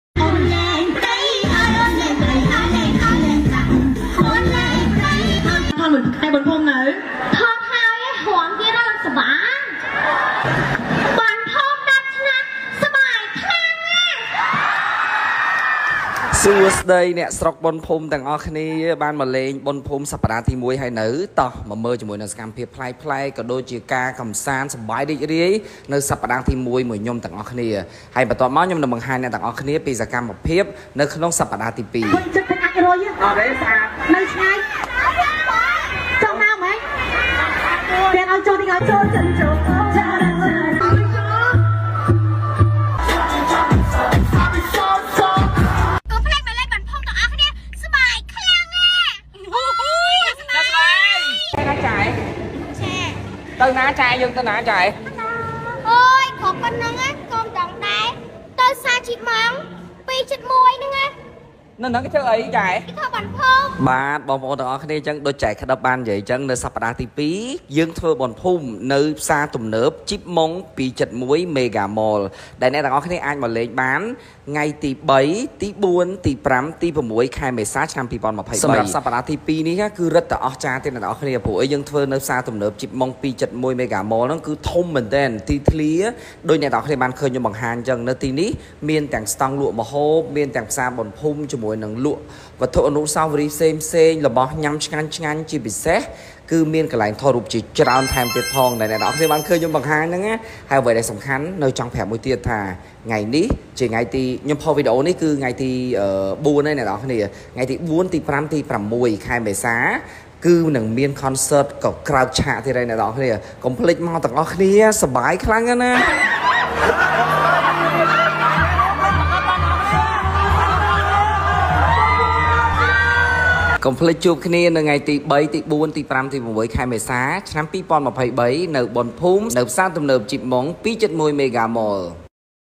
អូនក្តិបឡើងរាំបុណ្យភូមិបែបនារីខ្មែរ - គ្រួសារខ្ចៅខ្យង | Mr Rainniedara
ក្តិប ទៅជានារីខ្មែរ 🇰🇭

ចូលចិត្តទៅលេងបុណ្យភូមិណាស់ព្រោះសប្បាយ 😂♥️
សប្តាហ៍ទី 2 ធ្វើនៅផ្សារទំនើបជីបម៉ុង 271 Mega Mall